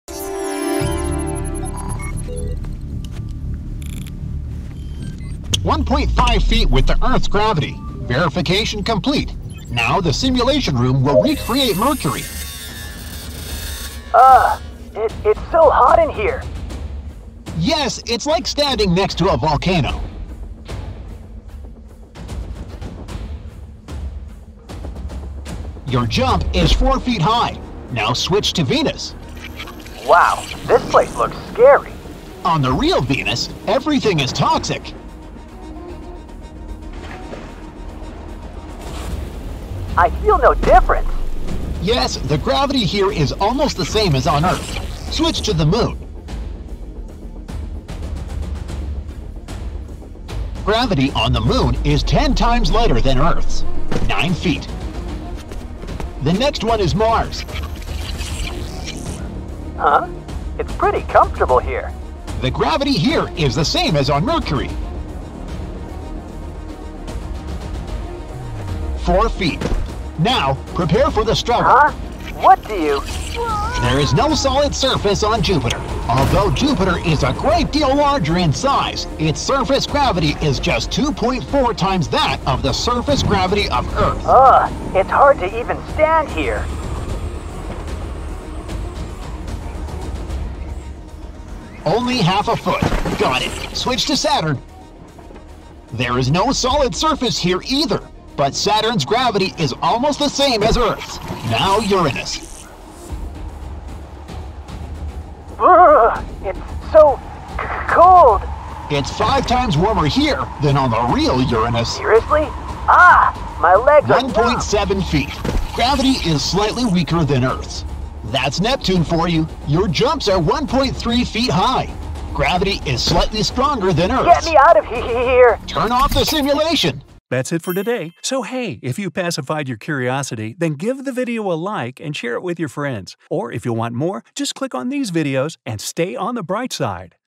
Gravity Simulation Comparison On Different Planets - 3D Animation
Buckle up - we're going on a journey to outer space. Don't worry - you don't need any special preparation as it will all happen inside a simulation room. Your mission today is to experience gravity on different celestial bodies and share your observations. Our first stop will be Mercury. Be careful, it's really hot here. But now it's time to move on to other planets!